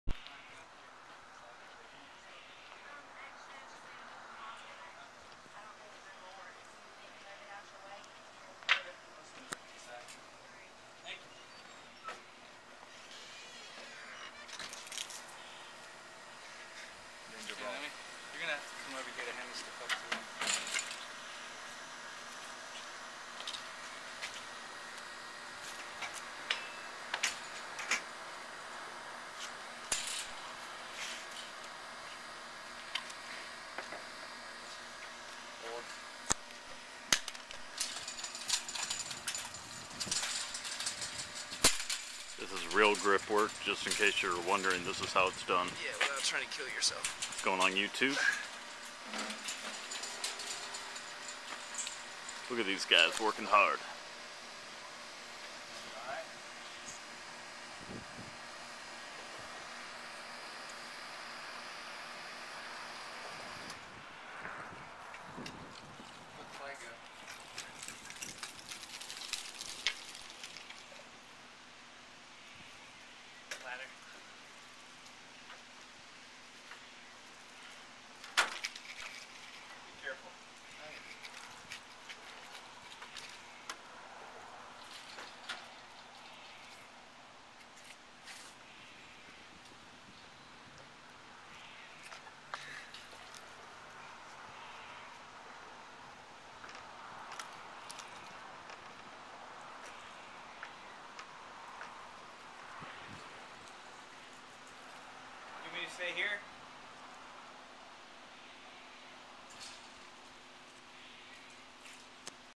Grips at Work
Behind the scenes on the set of "Swimming in a Lake of Fire".  A Highway 29 Motion Pictures & Vadini Productions feature film.